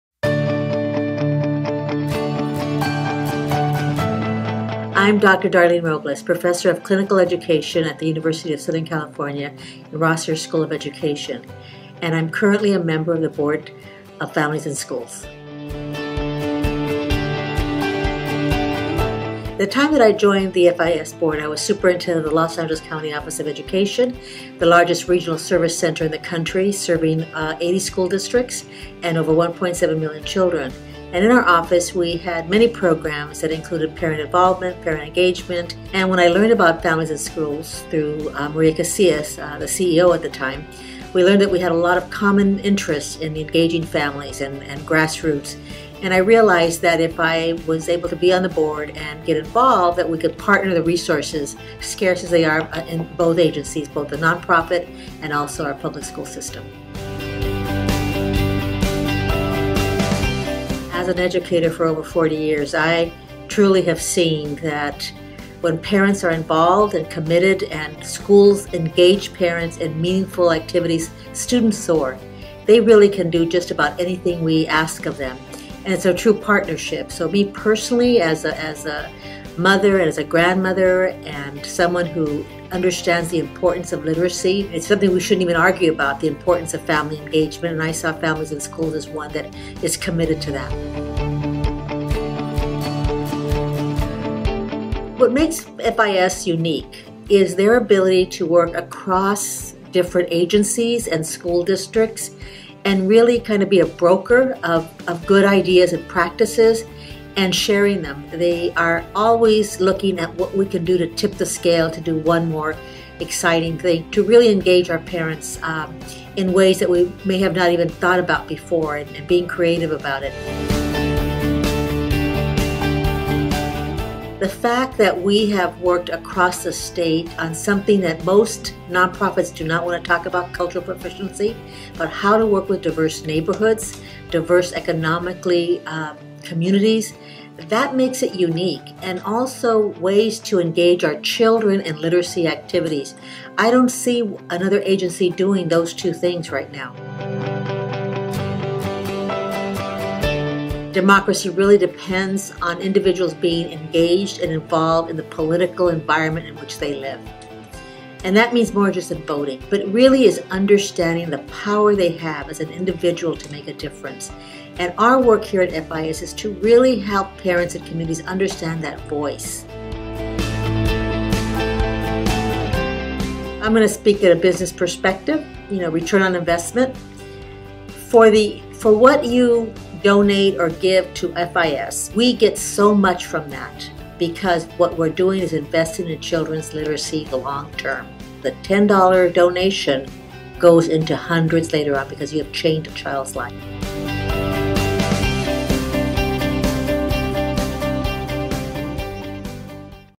A Conversation with FIS Board Member Darline Robles
Darline Robles, Ph.D., a professor of Clinical Education at USC, shares her involvement with - and enthusiasm for - the mission of Families In Schools, in this short interview taken at the FIS offices in the Pico Union section of Los Angeles.

Video produced by Paul Russell Laverack.